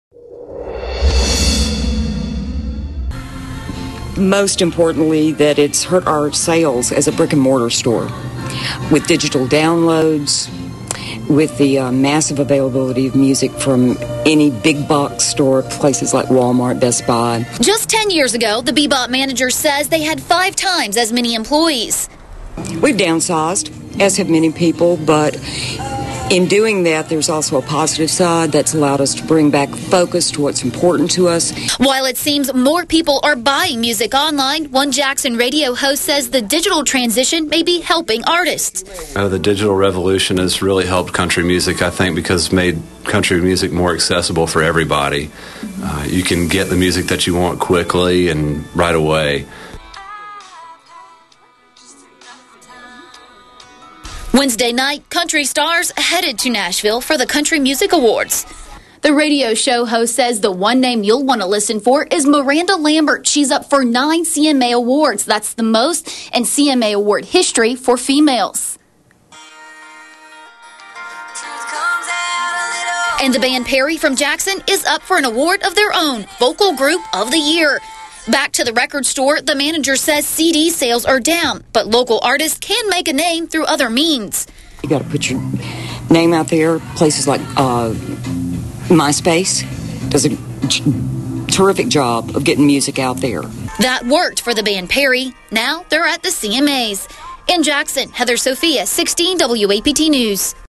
Digital Media Changing Music Industry
A transition to digital media is taking a toll on some aspects of the music industry.